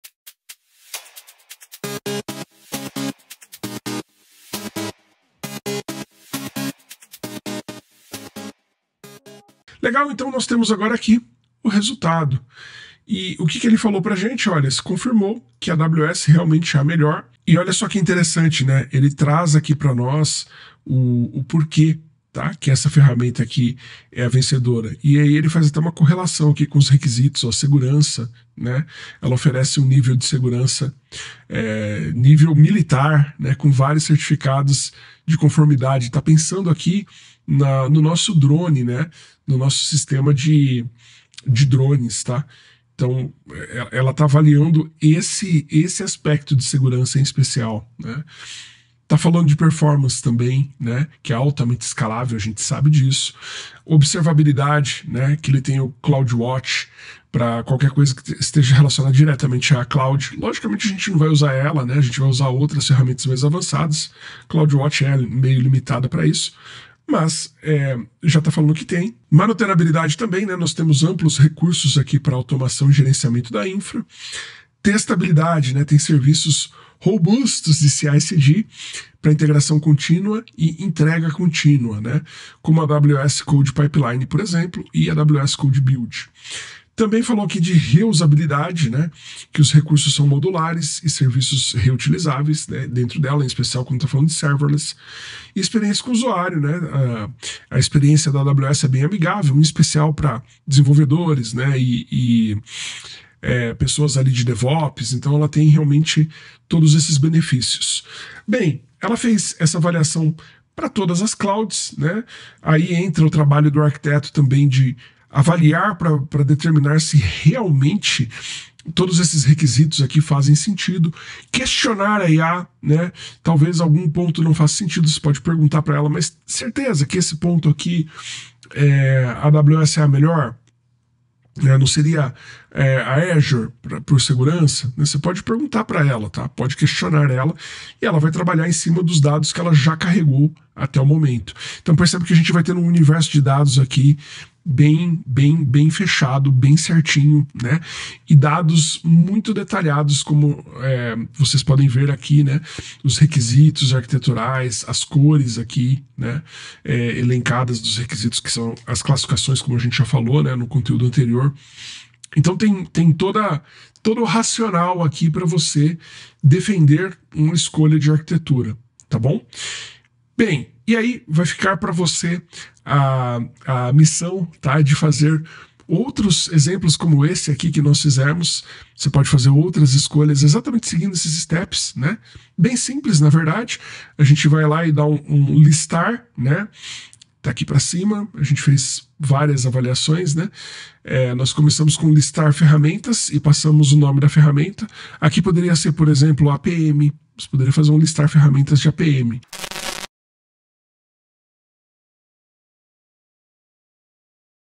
Decisões de Arquitetura com Tai
Neste vídeo, vamos ver a avaliação da Tai sobre as soluções de cloud, destacando a AWS como a opção superior para nosso projeto de drones. Discutimos os principais critérios que levaram a essa conclusão, como segurança de nível militar, escalabilidade, e uma experiência de usuário amigável, especialmente para desenvolvedores.

Assista a gravação da aula para mais insights!